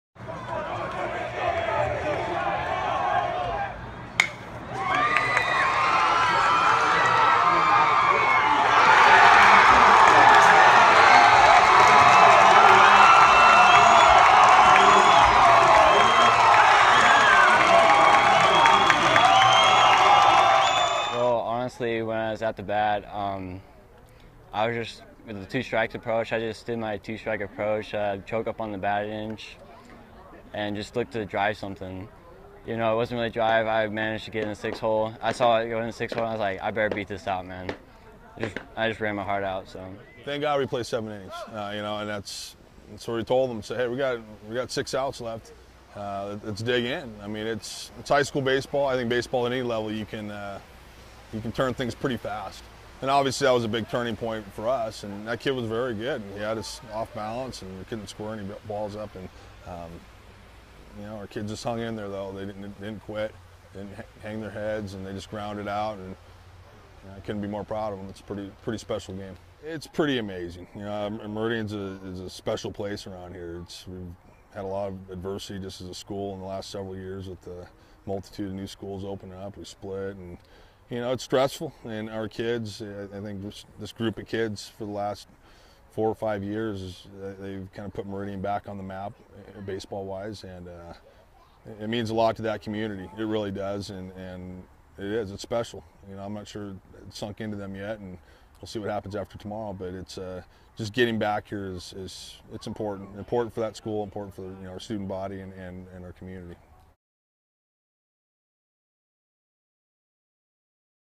Dibben's infield hit lifts Meridian into 5A state baseball championship
Pinch-hitter Bryson Dibben grounder scored the winning and the tying runs in a 5-4, come-from-behind win over Lewiston in the 5A state baseball semifinals. The victory puts Meridian into the Idaho state championship game for the first time since 1996.